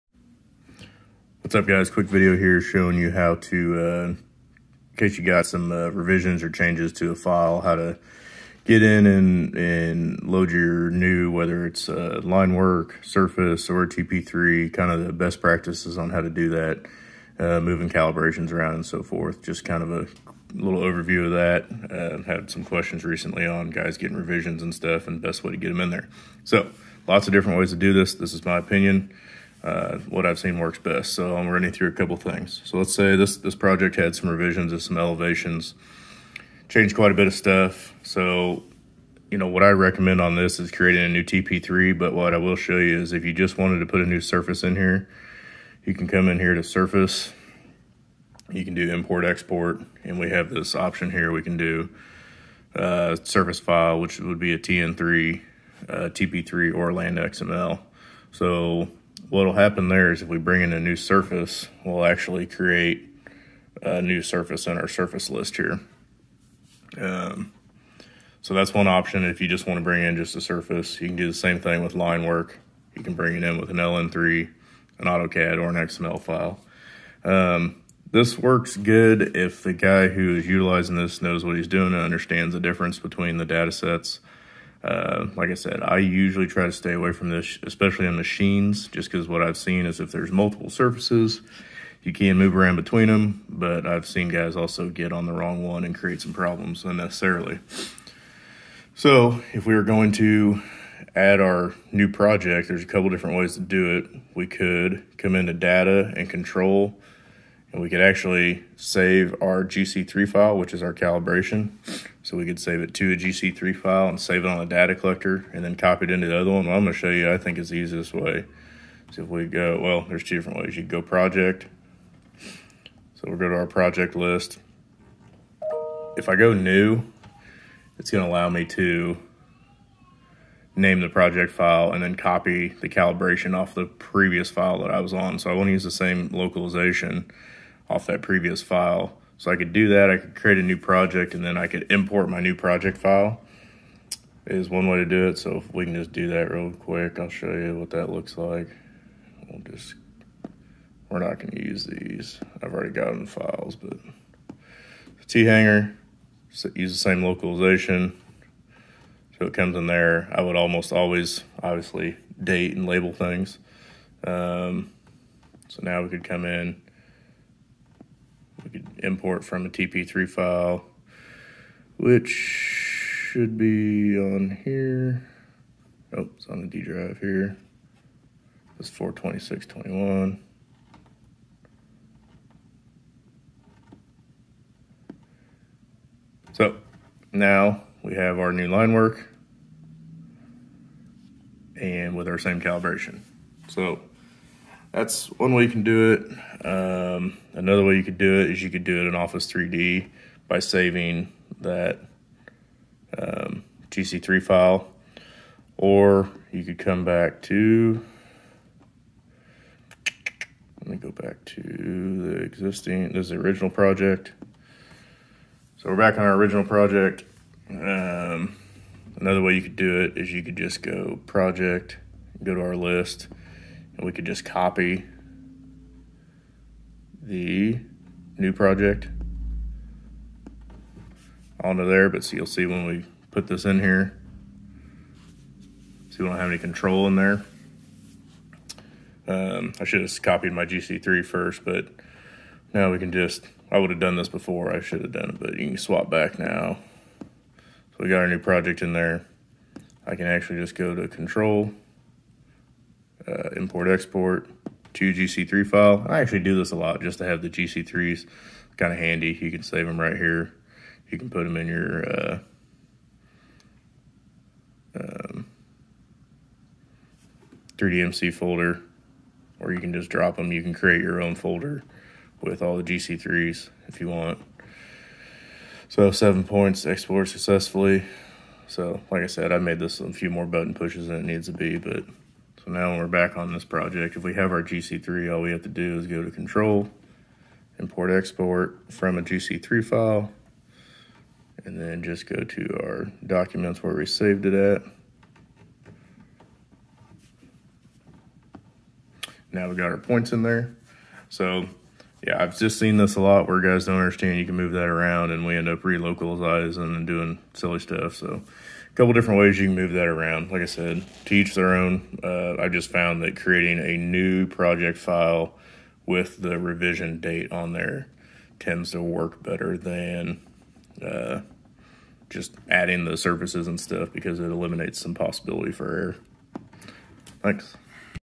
Copying Localization (.GC3) to new project in Topcon Pocket 3D.
How to create a new project using the same or original localization file in Pocket 3D.